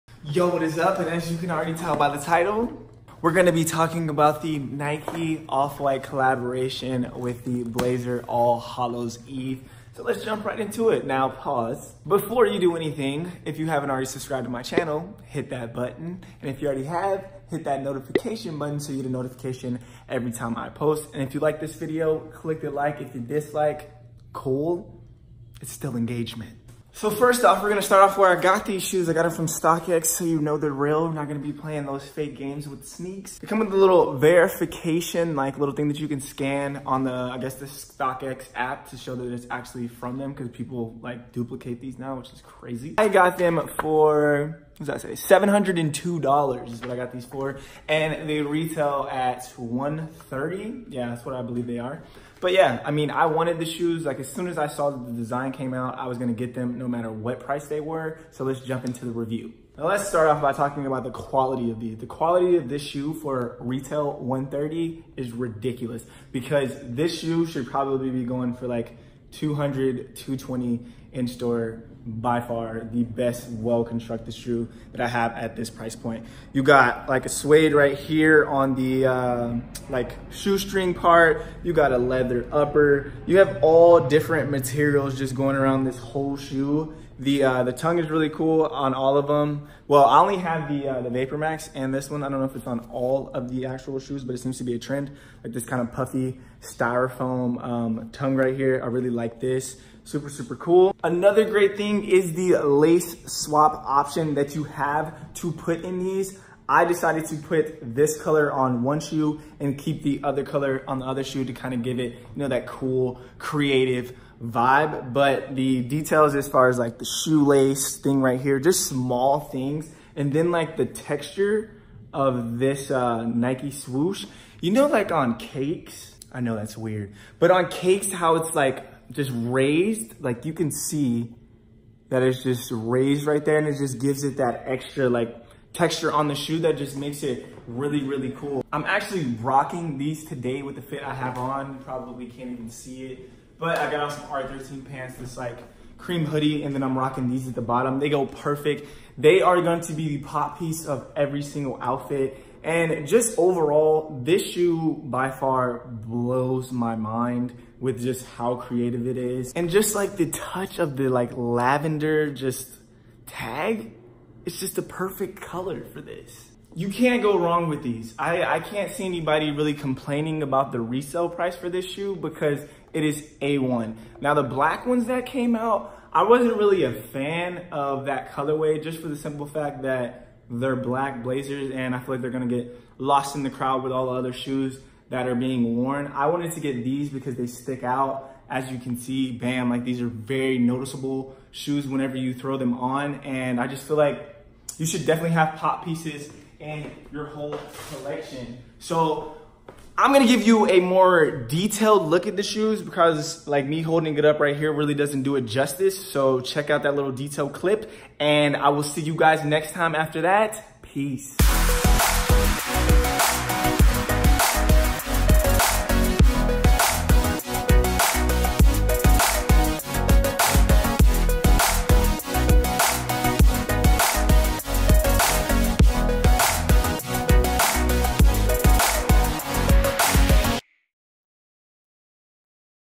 Nike Off White Blazer All Hallows Eve Review
Heres an in-depth look at my most recent shoe pick up. The Nike off-white blazer all hallows eve

Let me know if you guys got your hands on these and honest feed back. COP or DROP